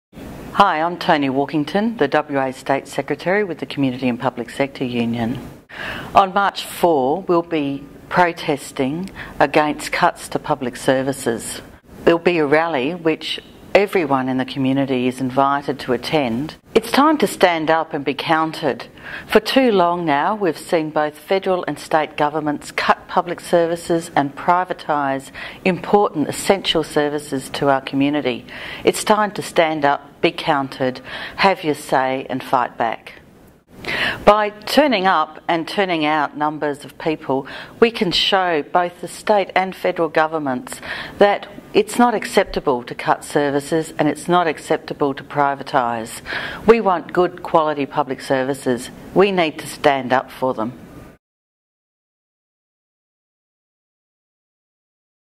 March 4 - Fight for your rights
Fight for your job and public services by attending the National Day of Action on the steps of WA Parliament on Wednesday,  March 4 from 12.30pm.

CPSU/CSA state secretary Toni Walkington says it is time to stand up and fight back as part of coordinated national protests across the country on that day.